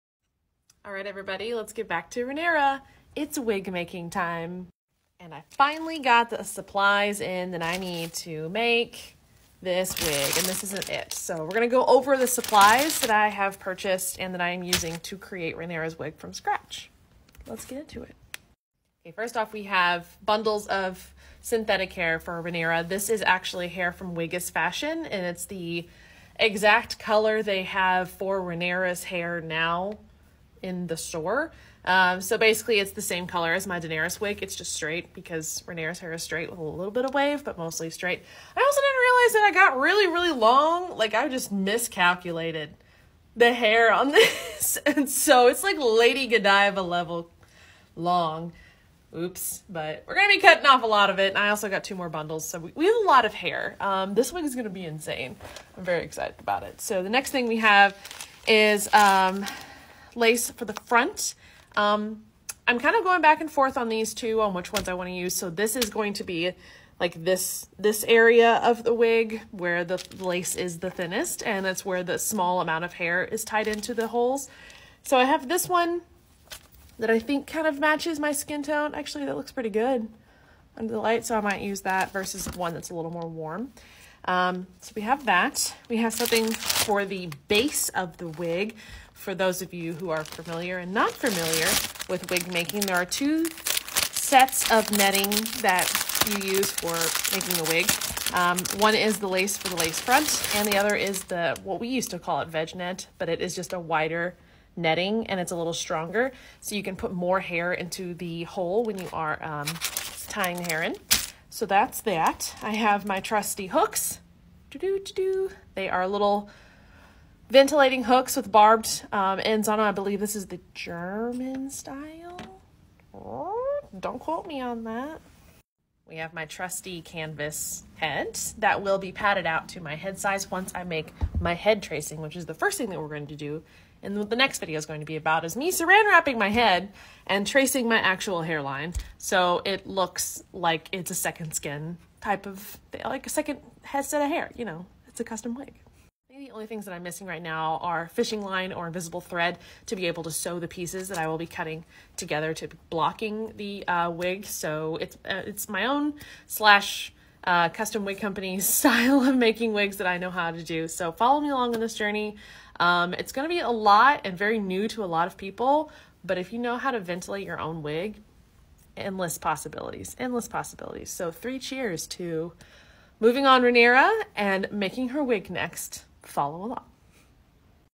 Wig-making starts now for Rhaenyra! | Making Rhaenyra Targaryen
Welcome to my Rhaenyra Targaryen series! I'm trying a different format, let me know what you think. Once I finish the cosplay, I will have traditional tutorials on Youtube. Enjoy this series!
We have an entire channel dedicated to this build.
MY BOOKS!

Threads - @jedimanda